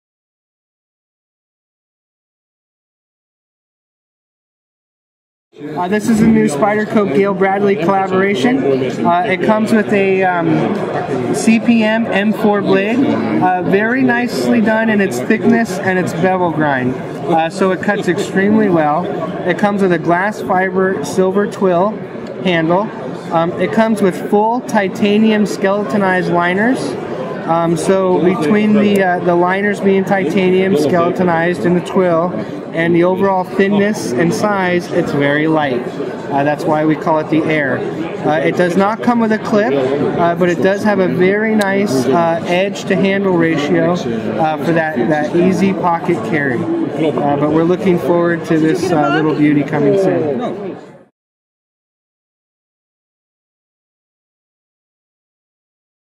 Spyderco 2012 Prototype - Gayle Bradley Air
Eric Glesser shows Spyderco shows and talks about Spyderco's new collaboration with custom knifemaker Gayle Bradley, the Air.